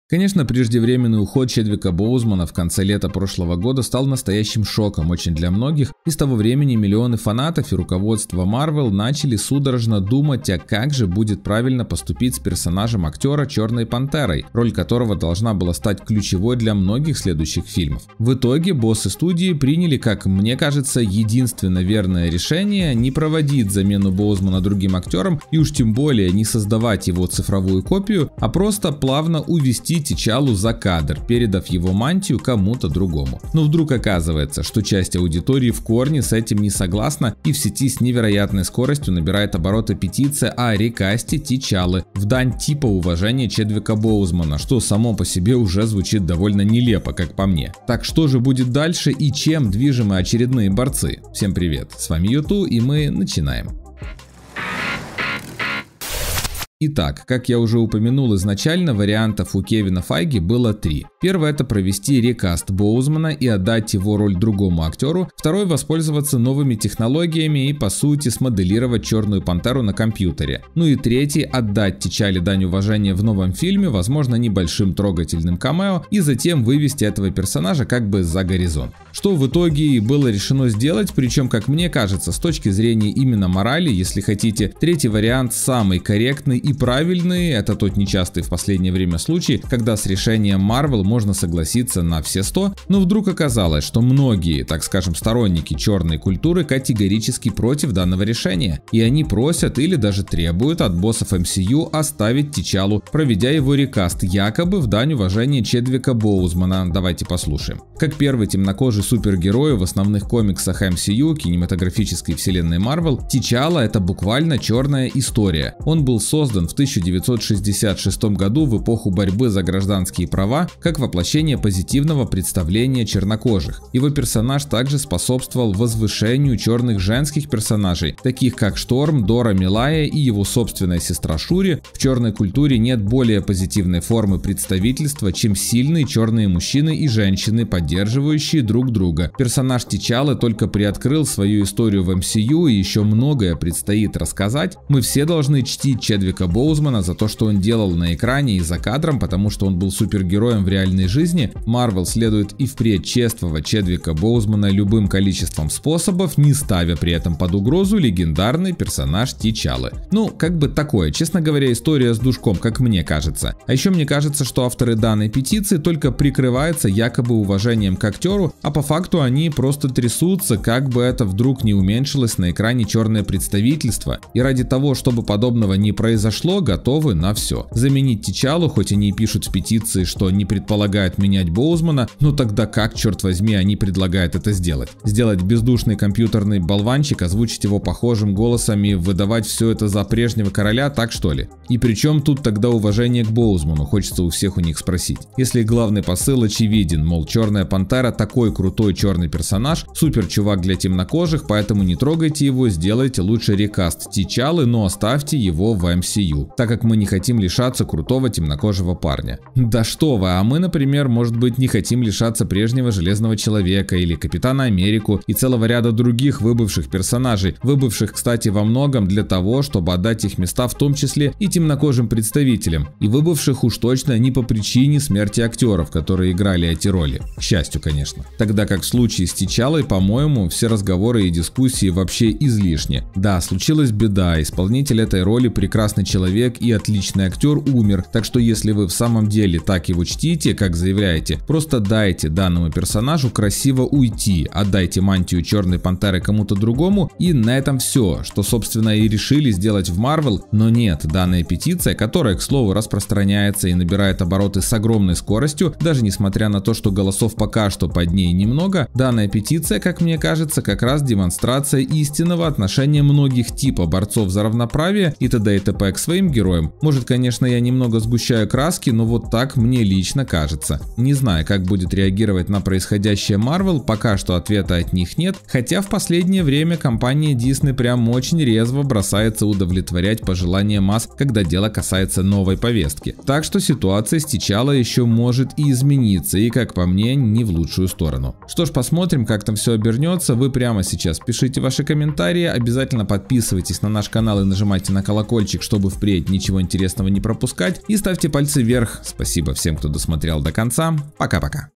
Жалкое лицемерие или дань уважения Чедвику Боузману? Что хотят борцы за "черную культуру" от MARVEL?
В сети активно распространяется требование не устранять Т`Чалу из Черной Пантеры 2, а сделать рекаст Чедвика Боузмана. И всё это, вроде как, в дань уважения Чедвику. Но так ли это на самом деле и что скрывается за новым требованием к Марвел?